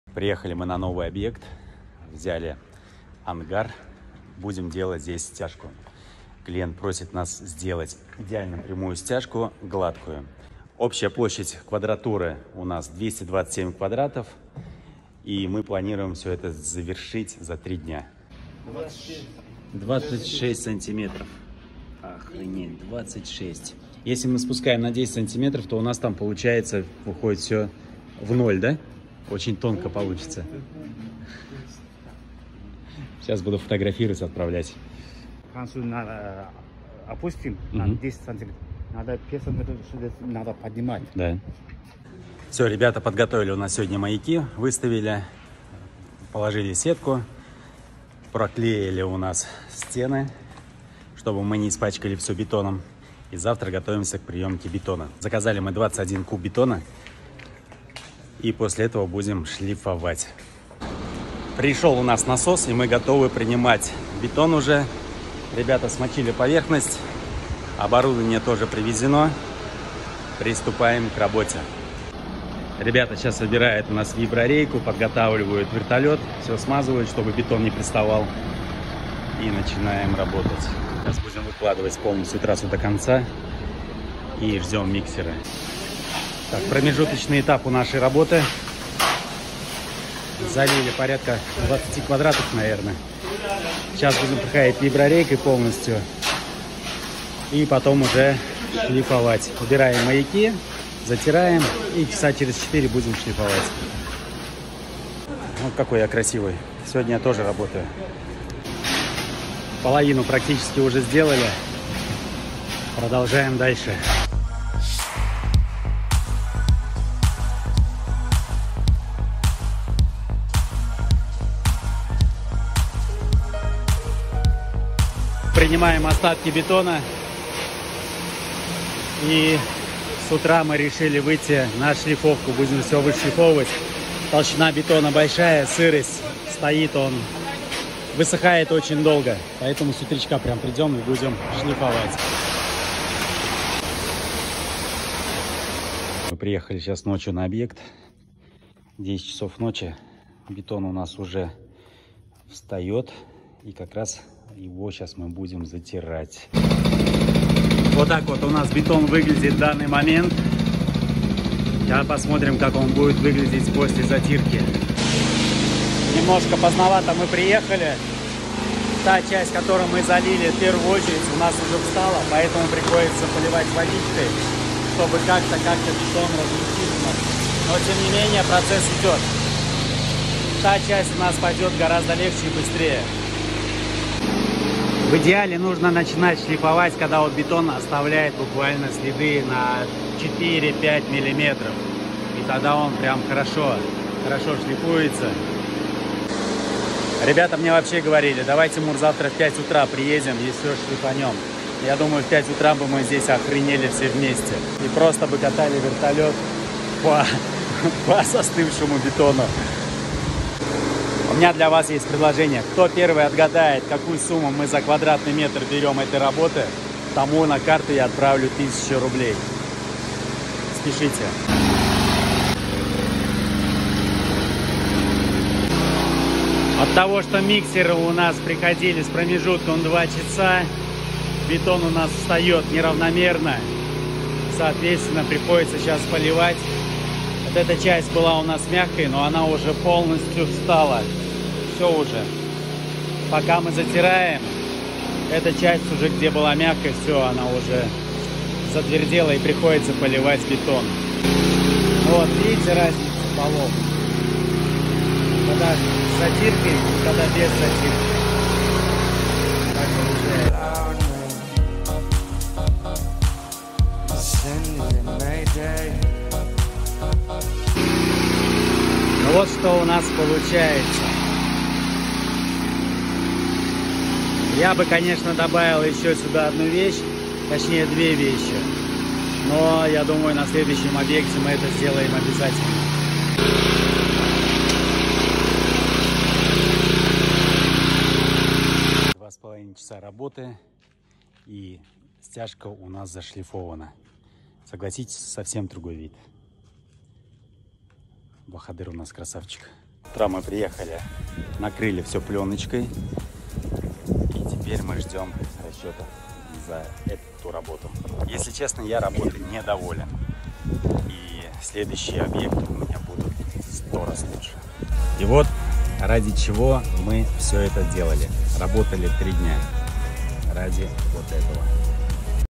Стяжка пола на складе площадью 218м2
Сколько денег можно заработать на стяжке пола за 3 дня? Пиши в комментариях, кто первый угадает тому 1000₽ на карту 😉